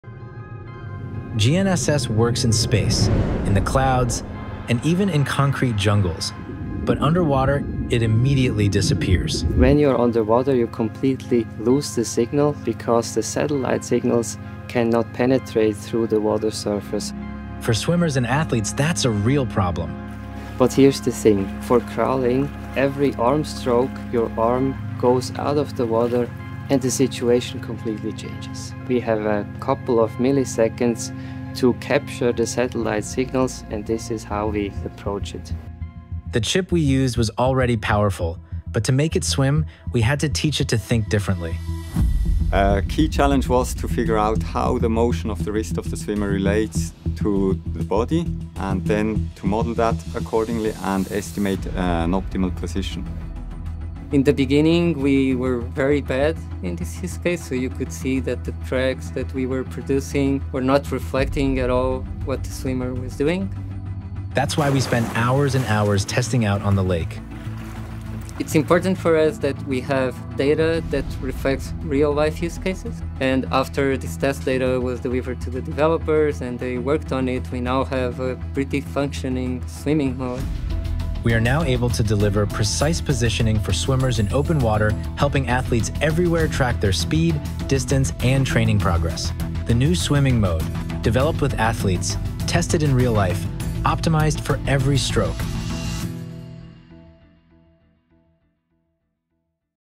GNSS that works while swimming | u-blox Swimming Mode
GNSS fails underwater - but u-blox found a way to make it work for swimmers. With u-blox Swimming Mode, swimmers can now enjoy accurate open-water tracking like never before. By capturing satellite signals during each arm stroke, this breakthrough mode delivers reliable data on position, speed, distance, and performance.

Tested with athletes. Built for real conditions. Optimized for every stroke.